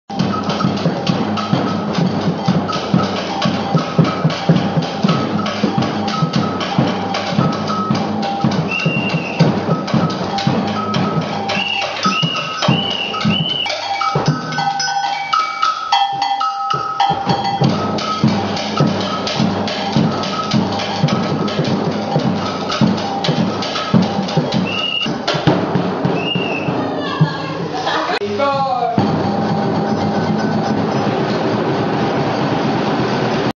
Samba course with Youth Against Settlements: summod & resistance center activities
أحد دروس السامبا في مركز الصمود و التحدي ، ضمن البرامج التدريبية في المركز